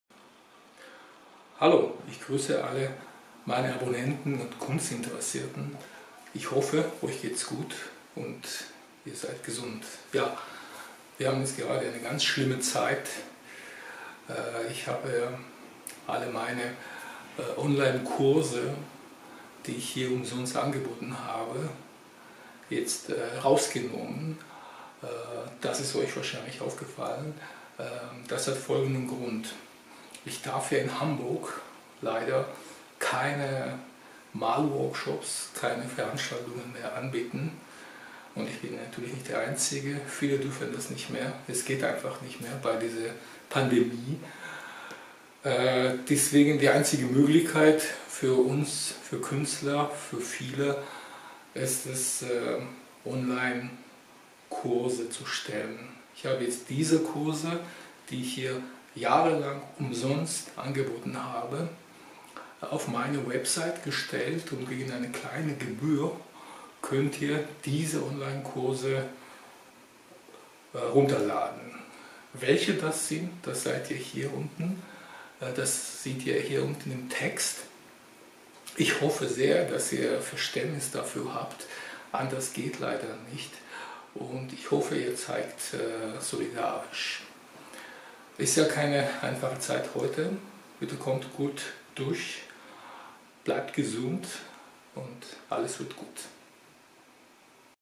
Onlinekurse in Coronazeiten Eine Ansprache (Online courses in corona times. A speech)
Liebe Freunde, liebe Kunstinteressierte,
aufgrund derzeitiger weltweiter Situation ist es mir nicht mehr möglich die Malworkshops zu veranstalten. Ich hoffe, dass wir die Lage bald wieder in den Griff bekommen und wieder zur Normalität zurückkehren.Bis das passiert, möchte ich euch auf meine Onlinekurse aufmerksam machen.Es sind einige Themen dabei, die ich in der Vergangenheit oft angeboten habe, einiges kommt noch in den nächsten Tagen oder Wochen hinzu.

Dear friends, dear art lovers,

Due to the current global situation, it is no longer possible for me to organize the painting workshops. I hope that we can get the situation under control again soon and return to normal.
Until that happens, I would like to draw your attention to my online courses.
There are some topics that I have often offered in the past, some will be added in the next few days or weeks.
Art is good for our soul and certainly helps us to survive the difficult time.

The following online courses are available (German with English subtitles):
Sgueegee technique